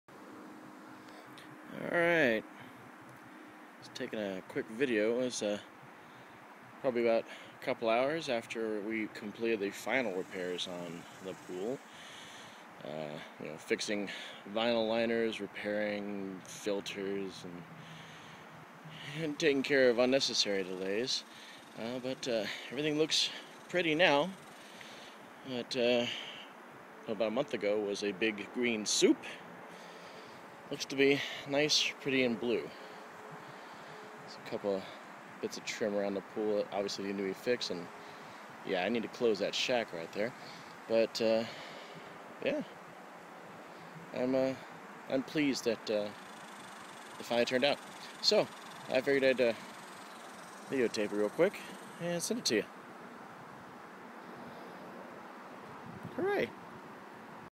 Pool aftermath
Quick vid of my pool after about 6 weeks of work. Originally green nasty water with a spotty filtration system bent on failure and a damaged liner.